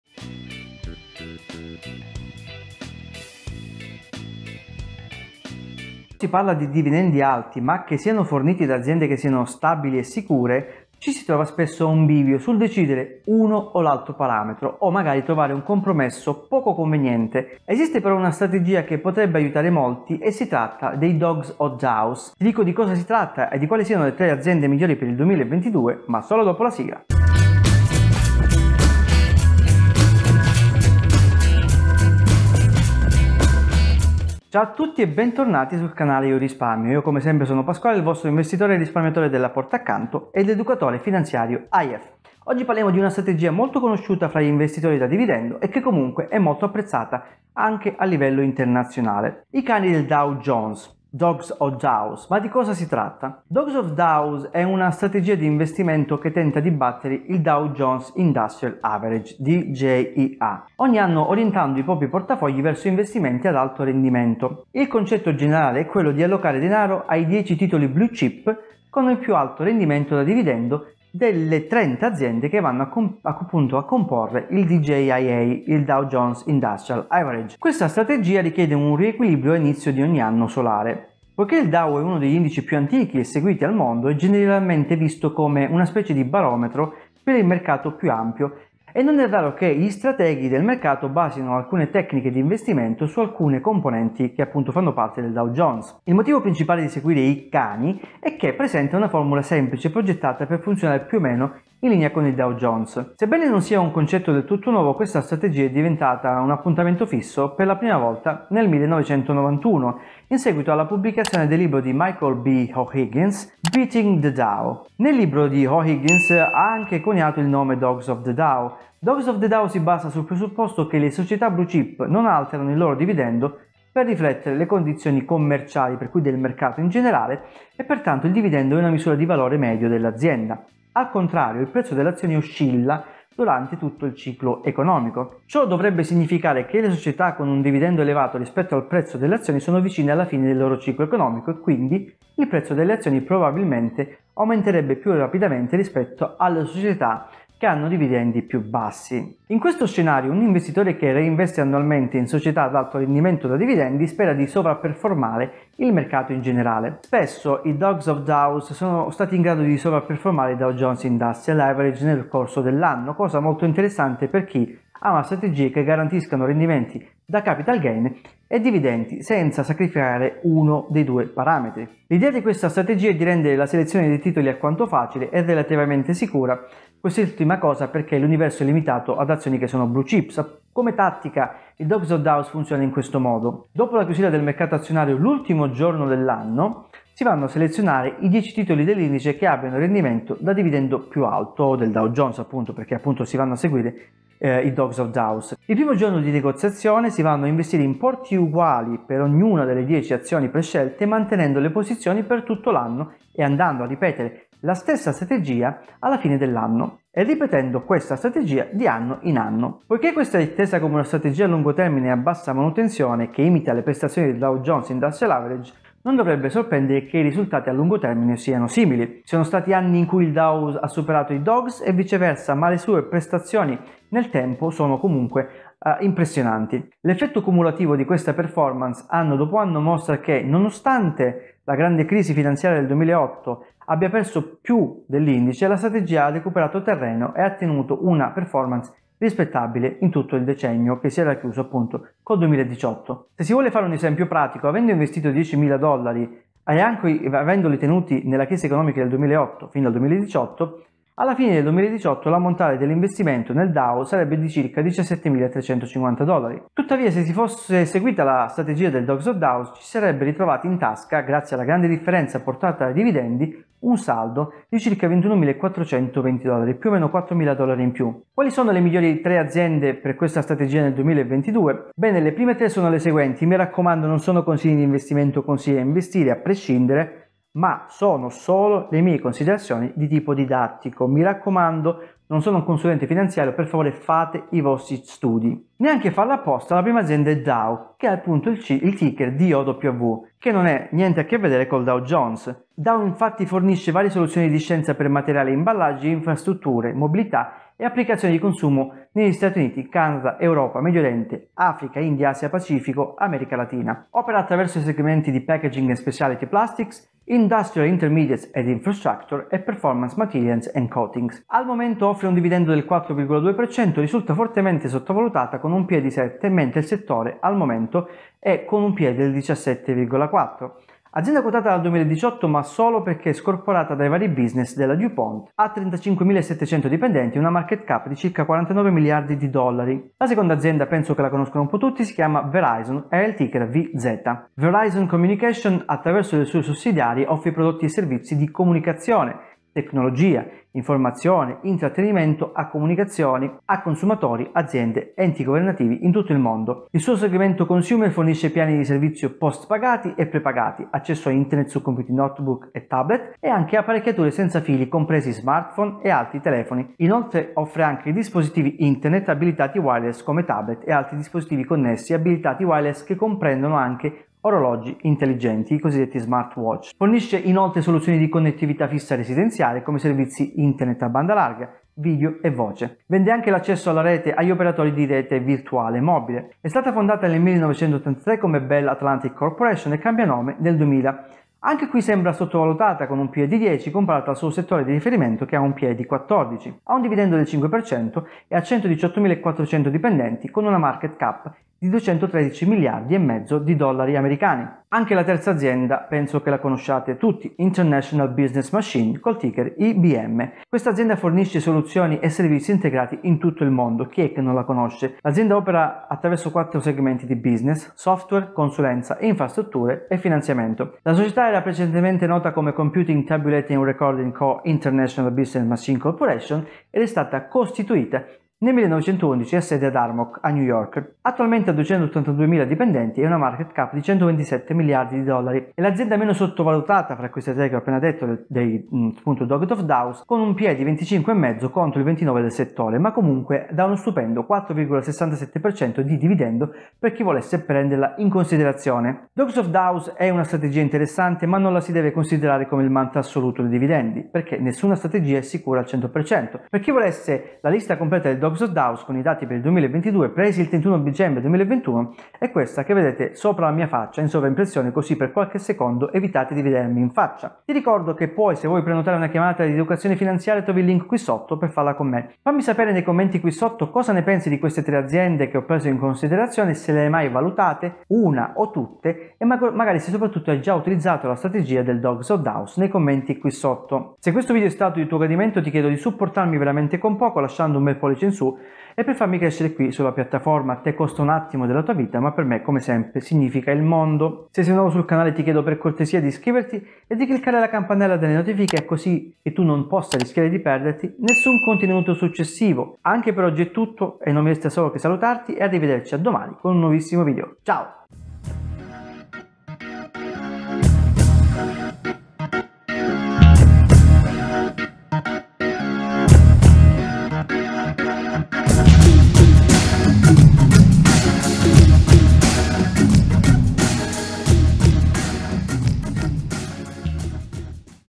I 3 migliori Dogs of Dow su cui investire nel 2022 - Strategia da dividendi vincente
Le tre aziende che menziono sono queste: DOW , VERIZON ED IBM

PEER TO PEER REFERRAL:

✅ EvenFi-Criptalia: EvenFi.com/iorisparmio